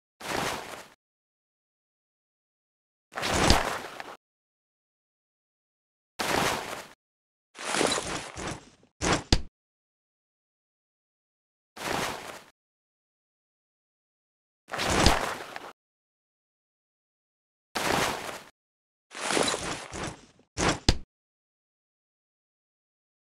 WiP Bottle Inspect Animations Render
These are animations I made for the bottle inspection stuff which is in the game's files, but is currently unused.
It's highly likely that we will be able to inspect custom bottles, bonesaws, gunslingers, etc this coming summer update.